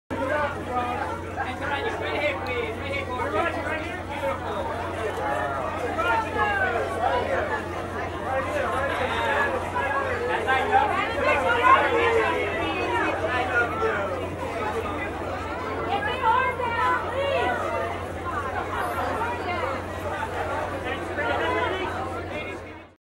Taraji P. Henson at 2017 Black Girls Rock
Taraji P. Henson attending the Black Girls Rock Red Carpet.